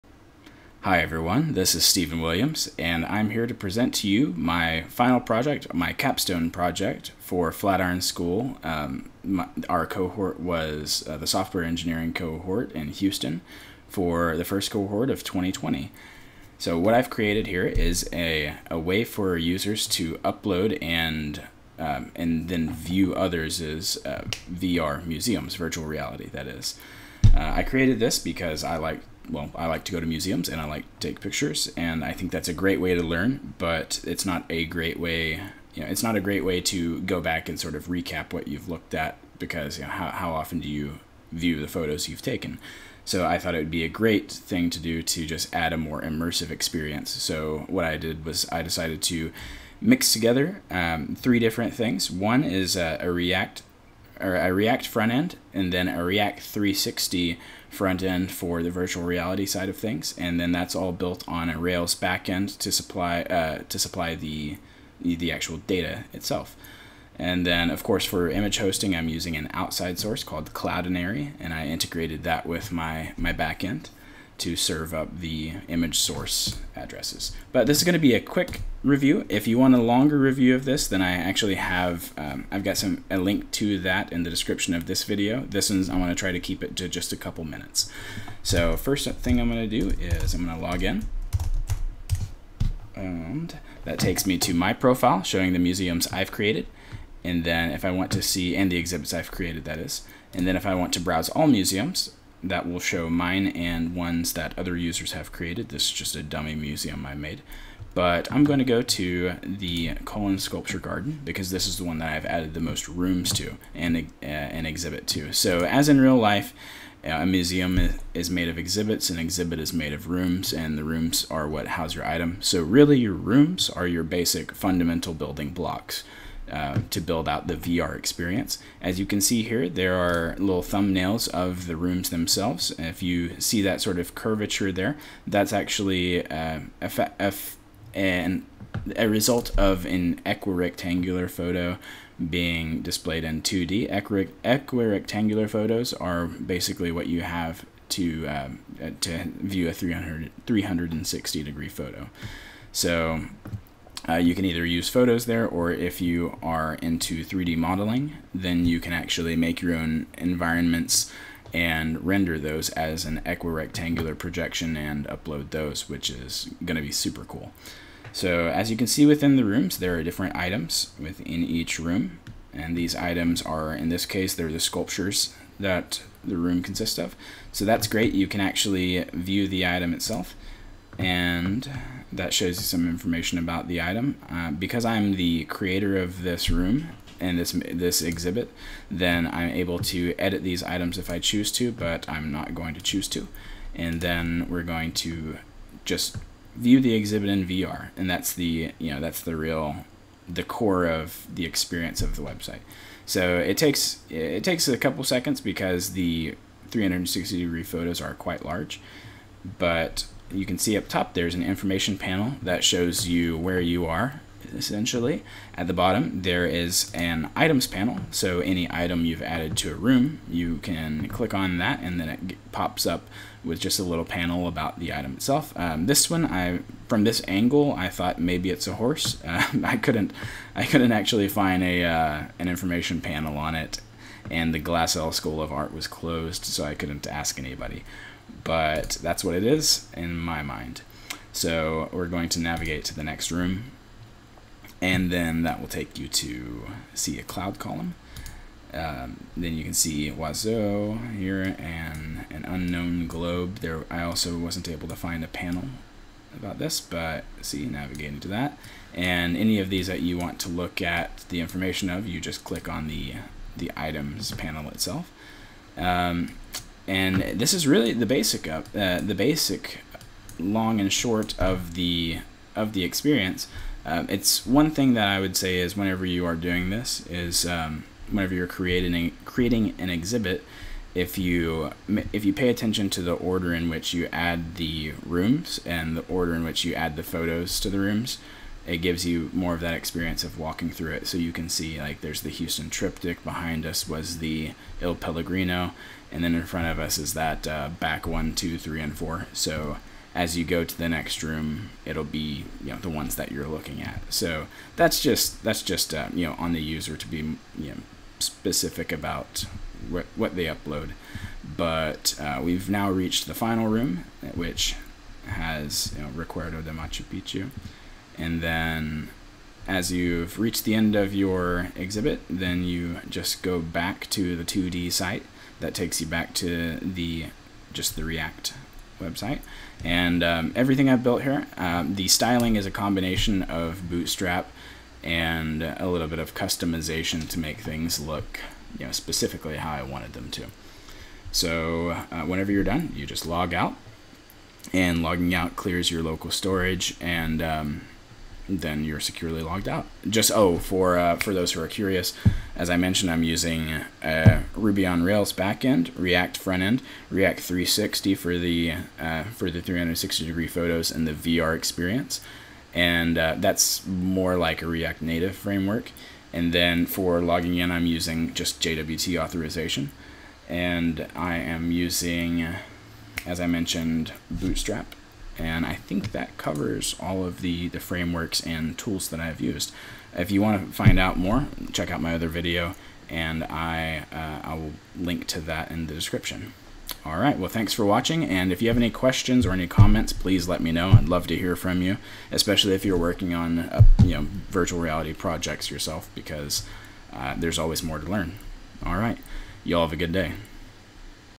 Muze 3D - Create and View VR Museums - Flatiron School SE Capstone
Here I demonstrate Muze 3D -- an application I made allowing users to create their own museums, complete with exhibits, rooms, and items. These museums can be viewed online in 2D mode, or they can be viewed in 3D mode on the browser.

The project is built on a Ruby on Rails backend, a React JS frontend, with a React 360 frontend for the VR site.

My LinkedIn profile is and I would love to hear from you there or in the comments below!